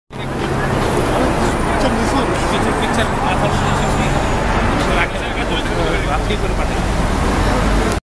Solidarity Rally in Philadelphia, PA
A solidarity rally to support Shahbag Movement in Bangladesh demanding capital punishment for war criminals from 1971 was held on March 24, 2013.